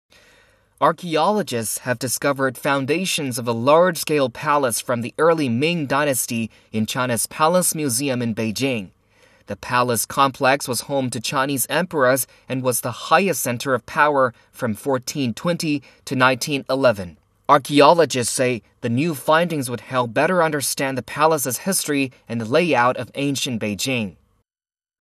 Foundations of large palace found
Archaeologists have discovered foundations of a large-scale palace from the early Ming Dynasty in China's Palace Museum in Beijing.

The palace complex was home to Chinese emperors and was the highest center of power from 1420 to 1911.p

Archaeologists say the new findings would help better understand the palace's history and the layout of ancient Beijing.